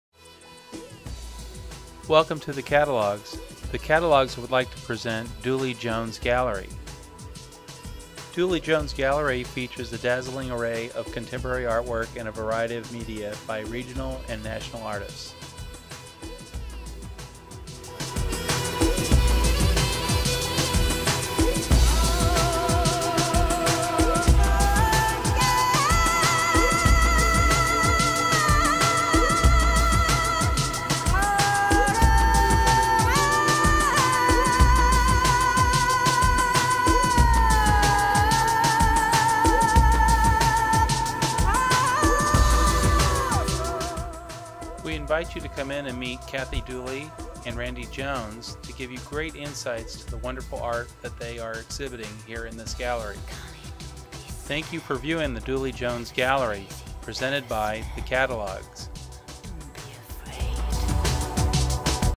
Duley Jones Gallery
Duley-Jones Gallery features a dazzling array of contemporary artwork in a variety of media by regional & national artists.  The eclectic selection includes the exquisite ceramic of George Alexander, the large watercolors of David Barba, Merrill Mahaffey's Grand Canyon paintings, Eric Boyer's wire mesh sculptures, and Robert Brubaker's whimsical figures. Paintings by Jean Richardson, Pat Bailey, Rhea Gary, Brian Blackham, Walt Wooten, George Green, Dick Phillips, David Gordiner, Marie Brumund, Marc Hatfield and Johnathan Harris are featured.